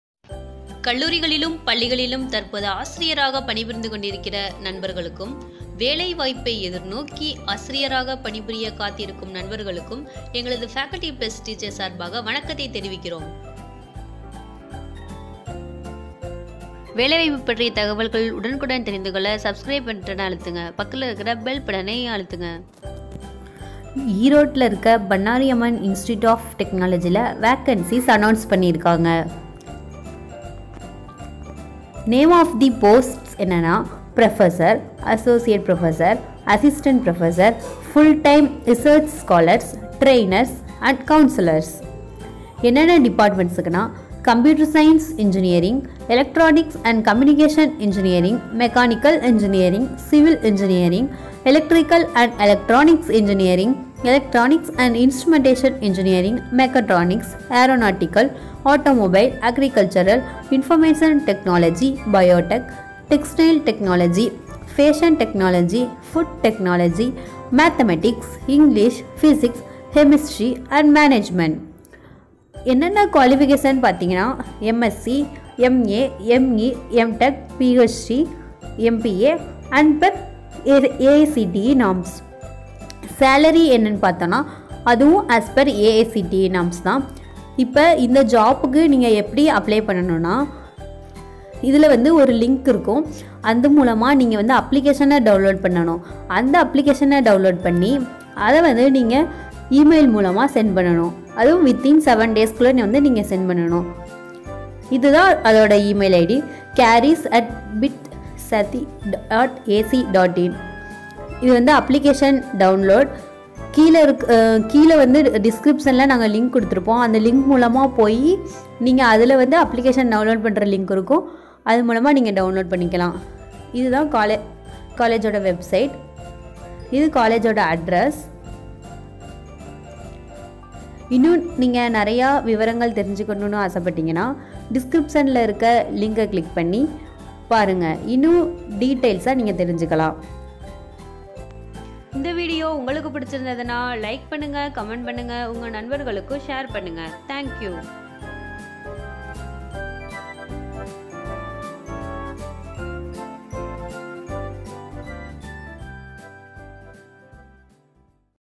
Bannari Amman Institute of Technology Erode Wanted Professor/Associate Professor/Assistant Professor
Bannari Amman Institute of Technology, Erode  has advertised for the recruitment of Professor / Associate Professor / Assistant Professor vacancies. Interested and eligible job aspirants are requested to apply within 7 days from 12th June 2018. Check out further more details in this video.